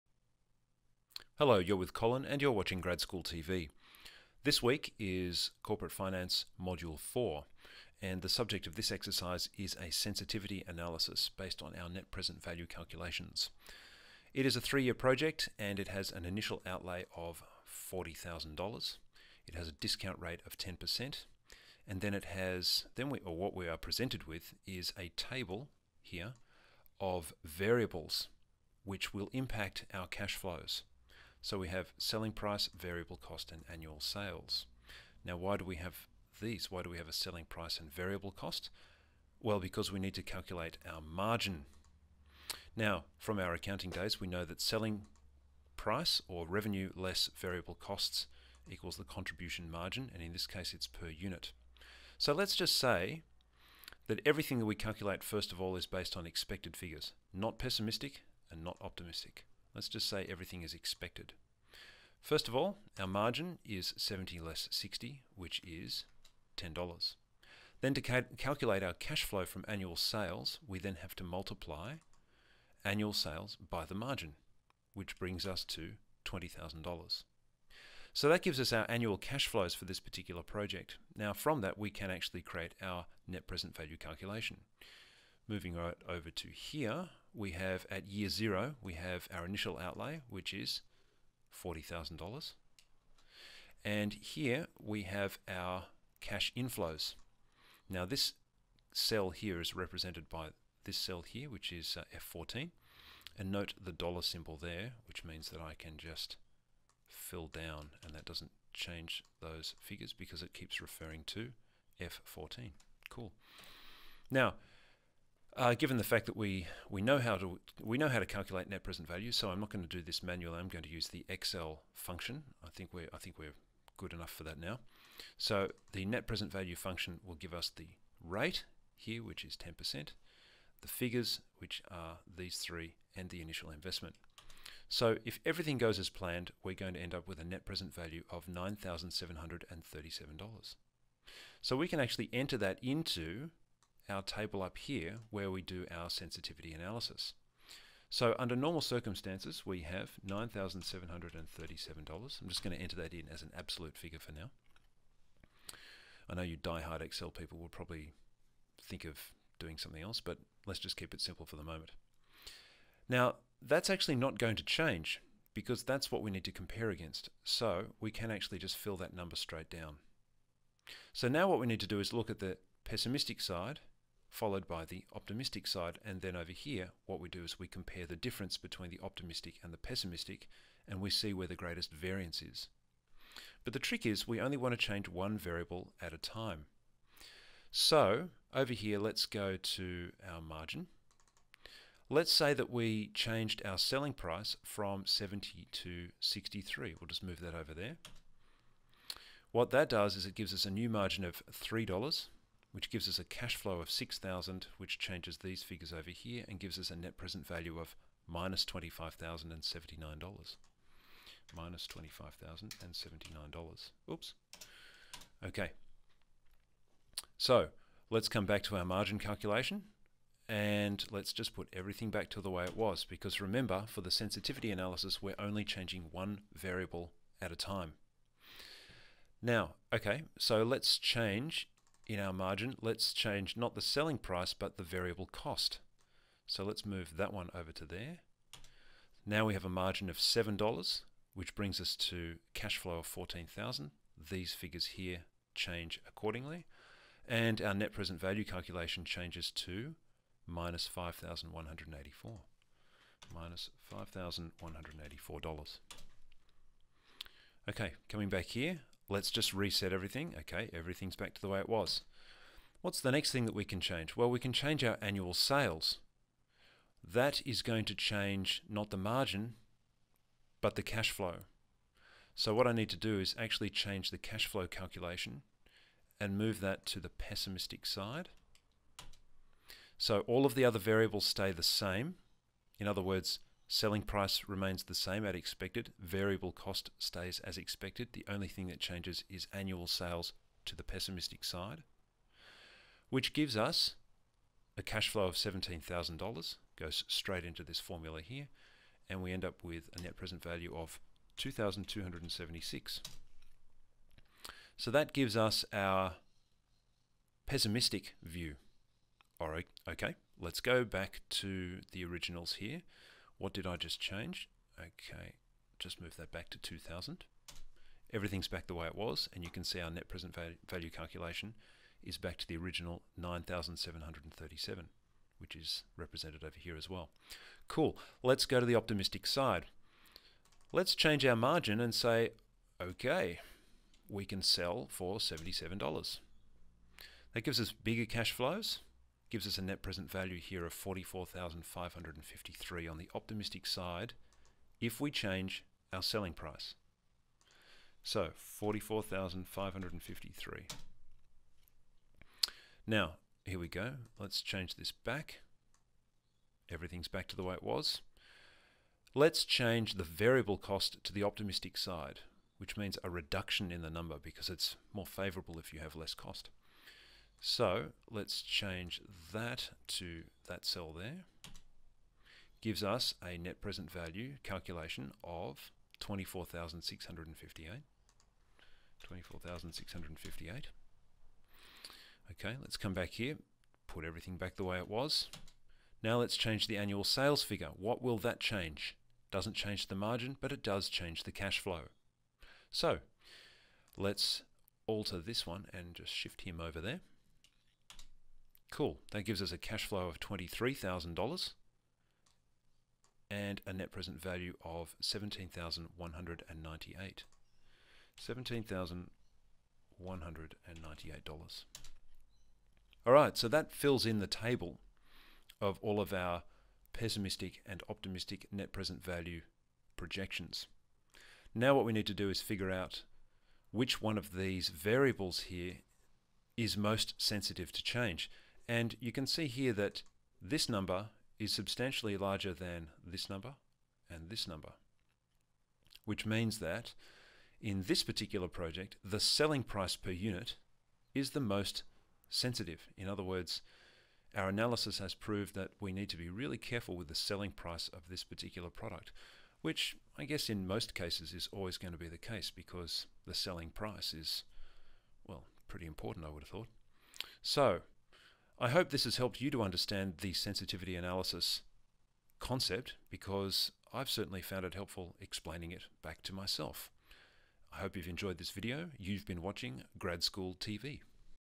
Corp Finance - module 4 - NPV sensitivity analysis
An example of a simple sensitivity analysis and how it affects the NPV calculation of a project.